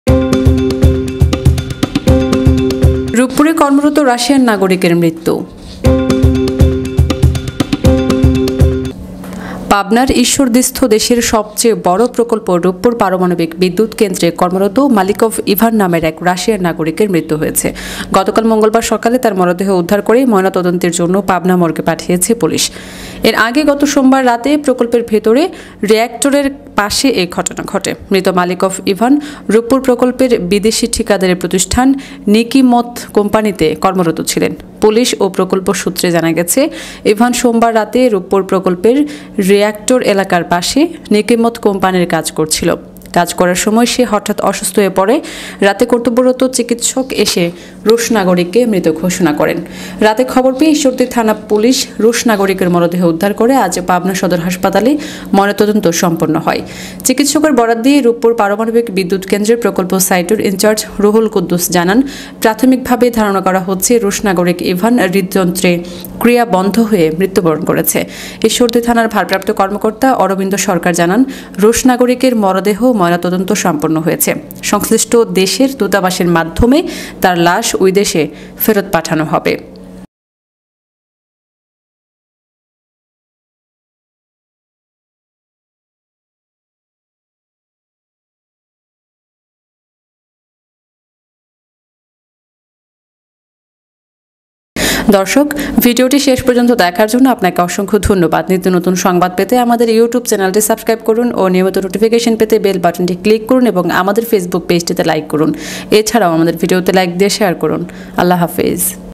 রূপপুরে কর্মরত রাশিয়ান নাগরিকের মৃত্যু | Ruppur Murder | Ruppur Nuclear Power Plant | Sheersha News
পাবনার ঈশ্বরদীস্থ দেশের সবচেয়ে বড় প্রকল্প রূপপুর পারমাণবিক বিদ্যুৎকেন্দ্রে কর্মরত মালিকভ
ইভান নামের এক রাশিয়ান নাগরিকের মৃত্যু হয়েছে। গতকাল মঙ্গলবার সকালে তার মরদেহ উদ্ধার করে ময়নাতদন্তের জন্য পাবনা মর্গে পাঠিয়েছে পুলিশ। এর আগে গত সোমবার রাতে প্রকল্পের ভেতর রিঅ্যাক্টরের পাশে এ ঘটনা ঘটে।

ধন্যবাদ..